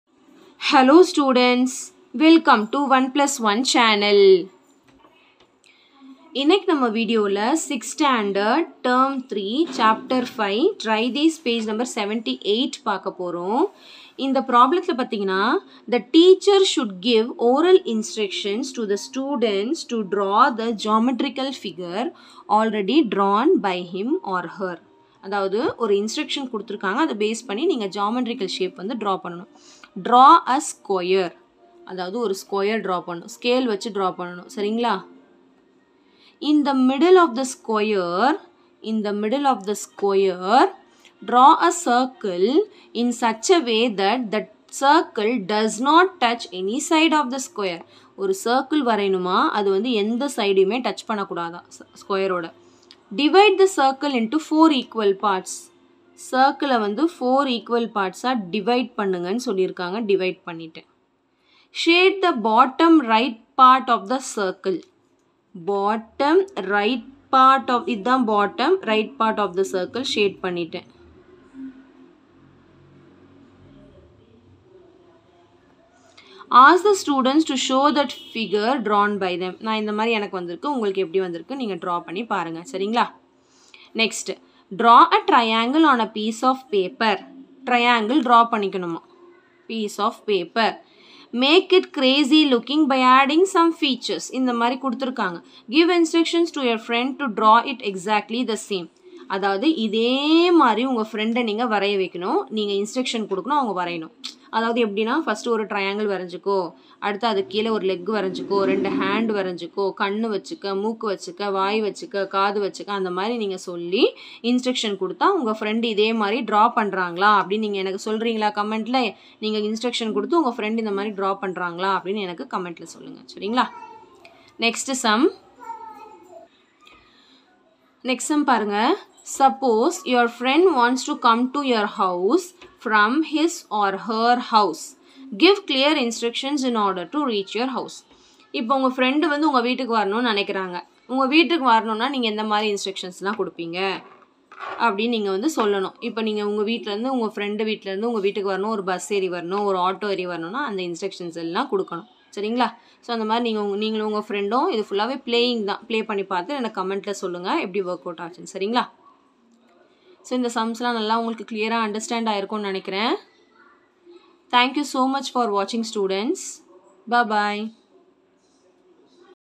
6th Maths T-3 Ch-5 Information processing Try these Pg.No.78 | Samacheer One plus One channel
This video contains for 6th standard maths Term-3 Chapter-5 Information processing Try these Page Number 78 complete solution.

My Another Channel 👇
Sangeetha Media